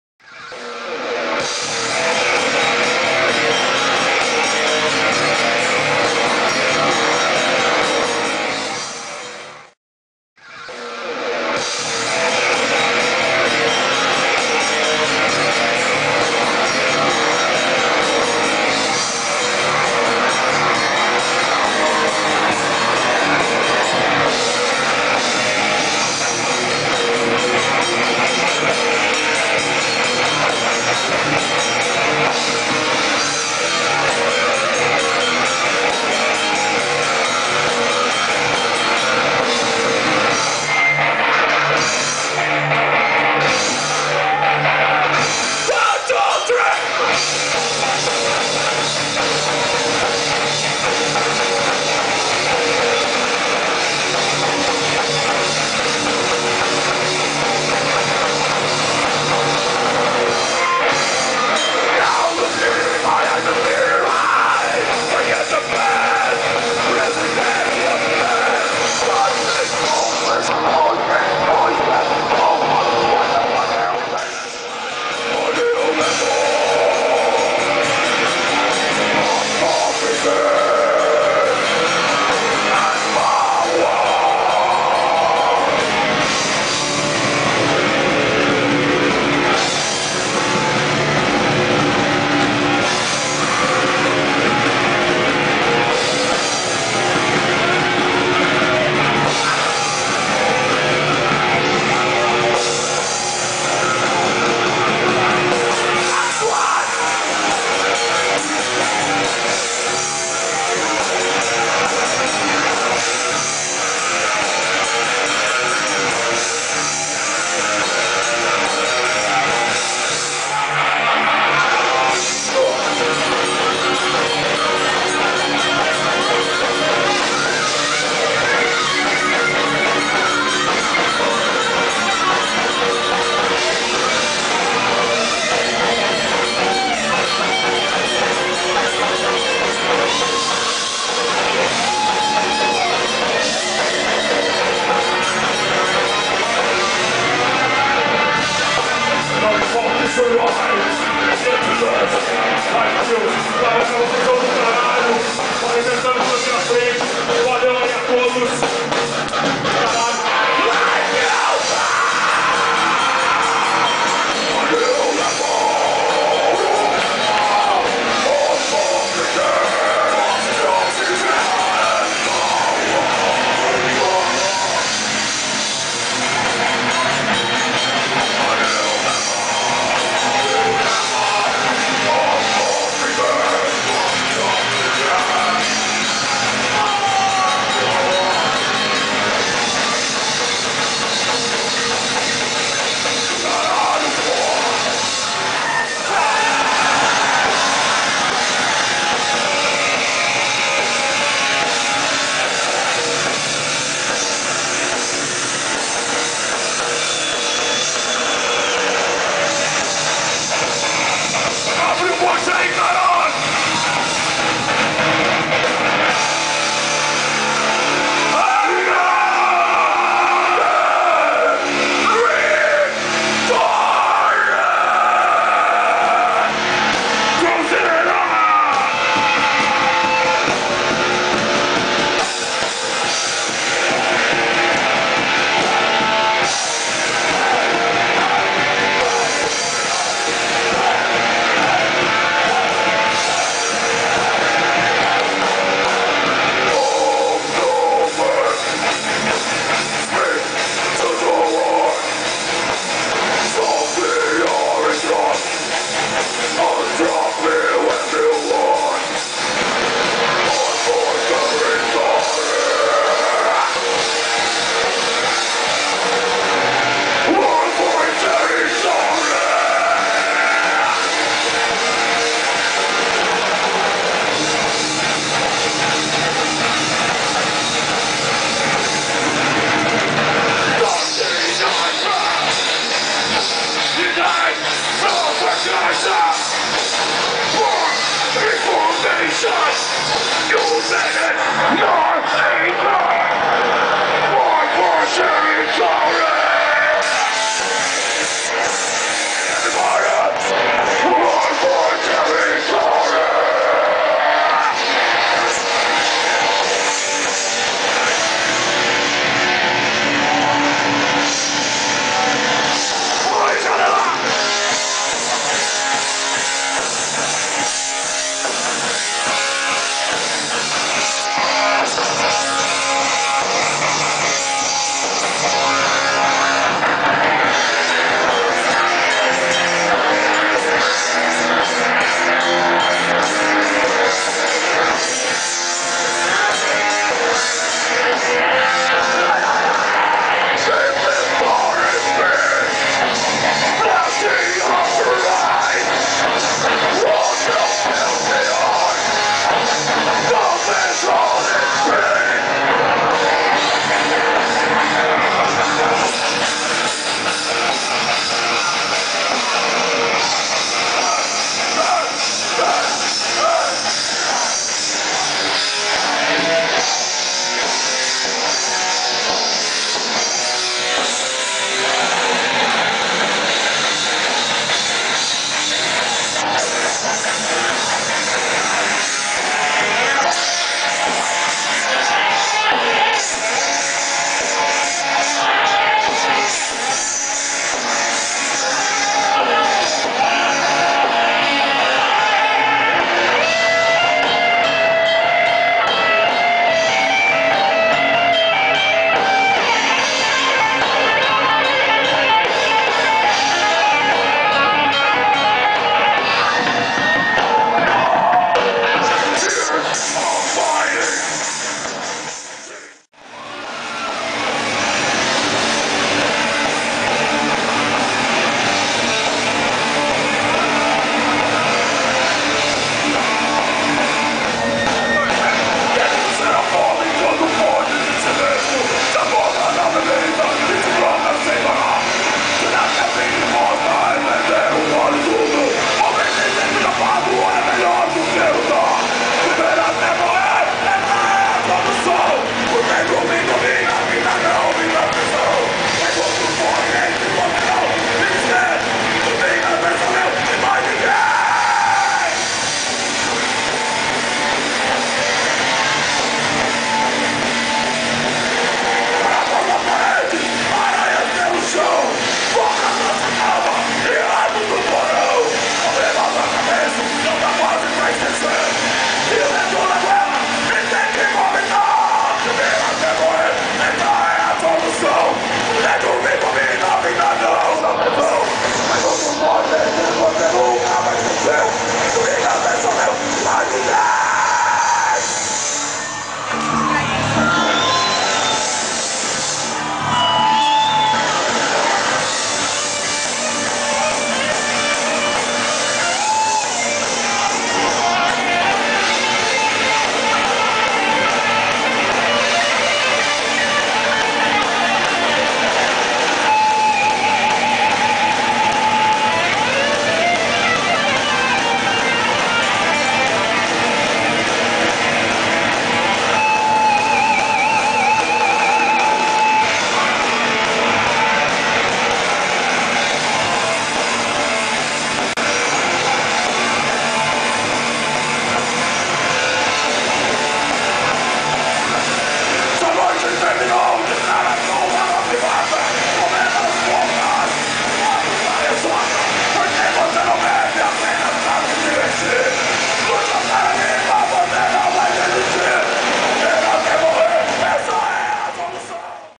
cavera no campus of rock pt2
CAVERA thrash metal band, no festival CAMPUS OF ROCK, realizado no d.a da feg no dia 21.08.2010.